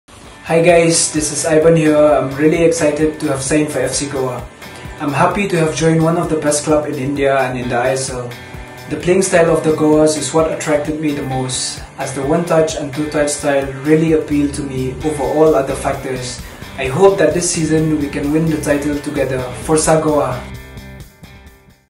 Aiban Dohling First interview
Aiban is all set to don the FC Goa jersey! Listen to what our new Gaur had to say about his new journey in the ISL- Indian Super League! 🔥